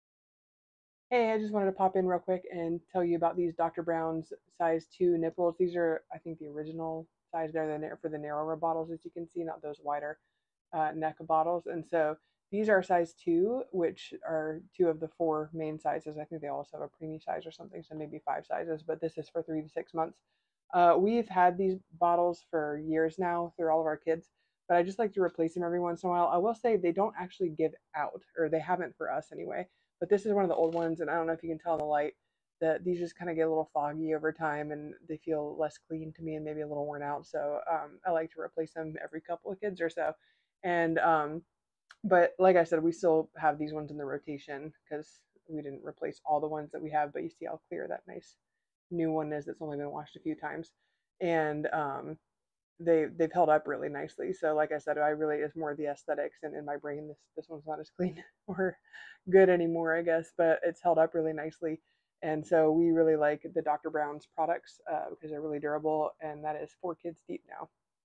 Review of the Dr. Brown’s Natural Flow Level 2 Narrow Baby Bottle Silicone Nipple, Medium Flow, 3m+
I only post reviews for products that I own, use, and love! I appreciate you!

"100% Silicone Bottle Nipple, 6 Count"
ASIN for this product: B015XBYDLM